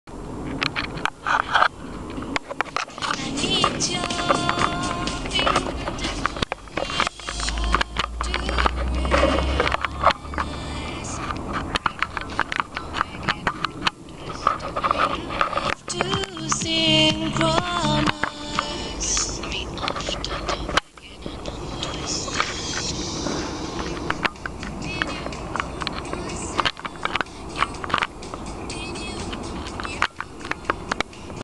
KASTORIA - EASTER VACATION APRIL 2009
A DESPERATE AND DIZZY EASTER, KASTORIA IS AN ABSOLUTE PARADISE, MYKONOS IS NOT.
I HAD A POOR FISHING OVER THERE, BOTH IN THE LAKE AND IN THE CLUB. HAHA... AND SORRY TO SAY THAT THOUGH KASTORIA HAS BEAUTIFUL FUR PRODUCTS BUT MOST OF BITCHES ARE NOT AS GOOD AS ACROTIRI ONES, THEIR SHAPES LIKE A JAR CAN.